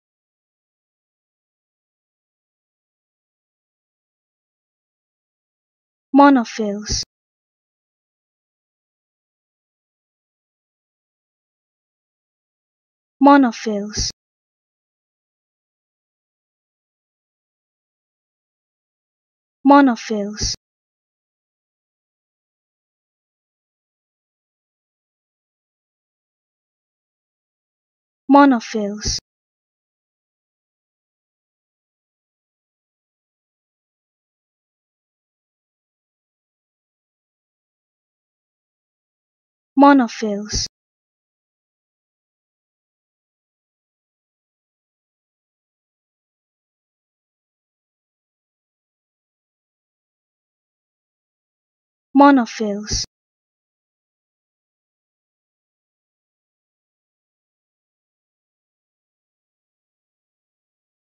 How To Say Monofils
Learn how to say Monofils with EmmaSaying free pronunciation tutorials.